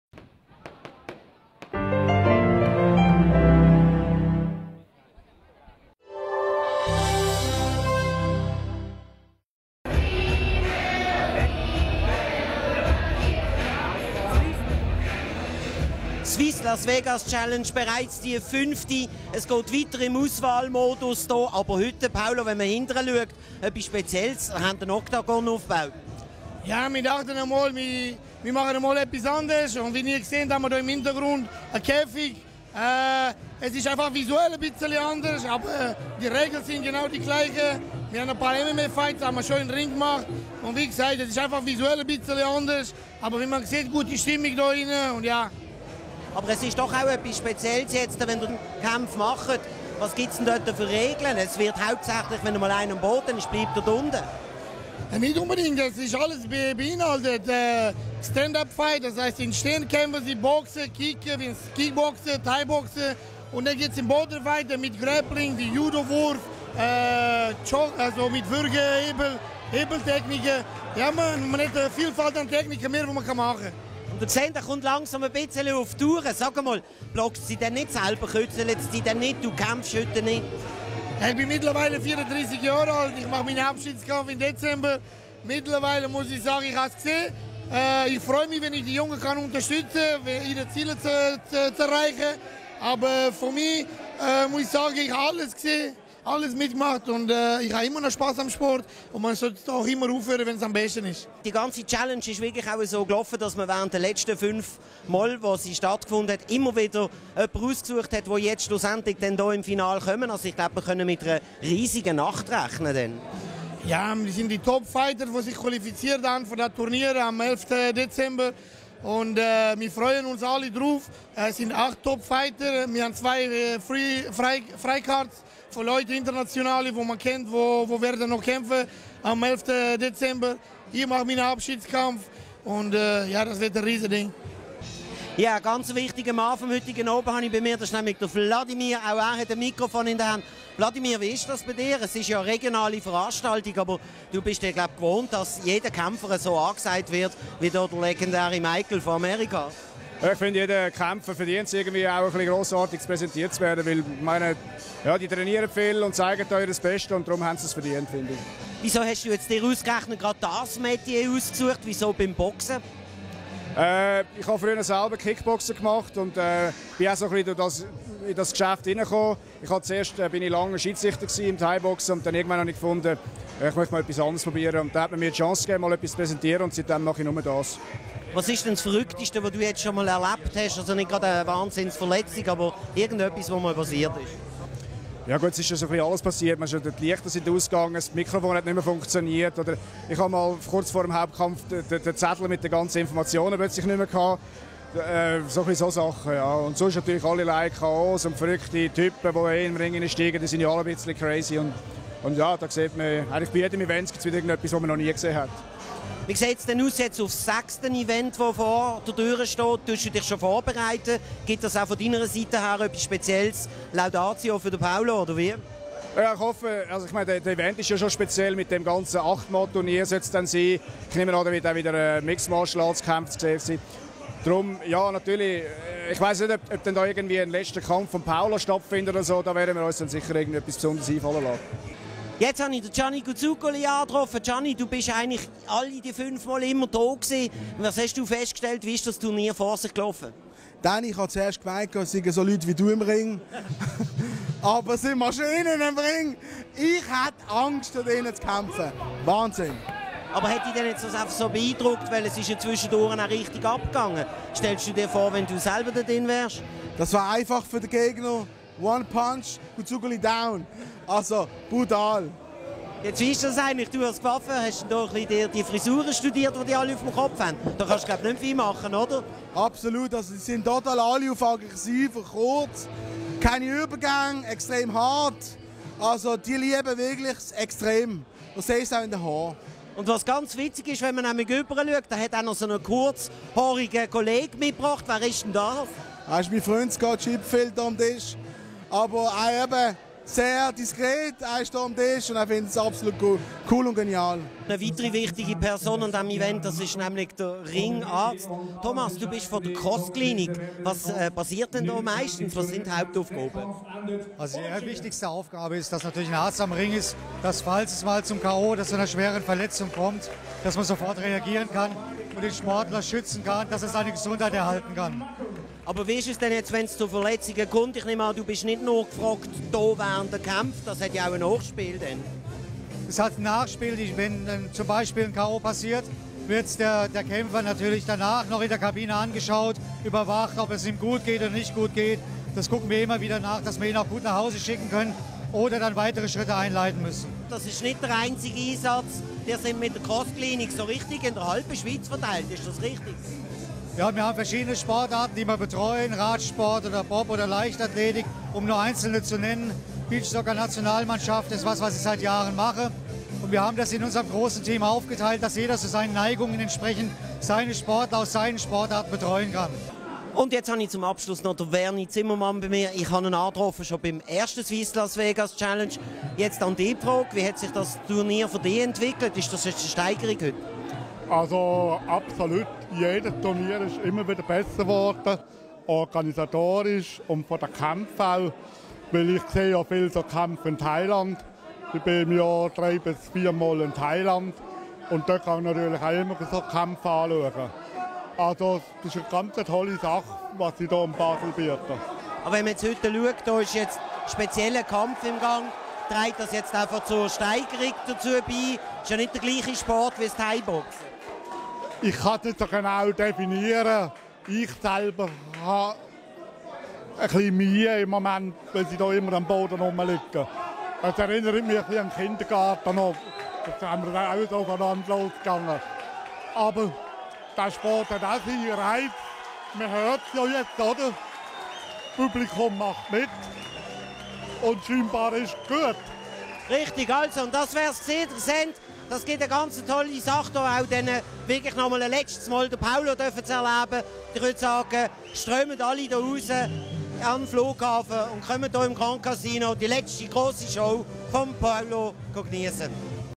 Swiss Las Vegas Challenge 2011 - regioTVplus
Martial Art im Grandcasino de Bale
Die 5. Ausgabe ganz im Zeichen des Oktagon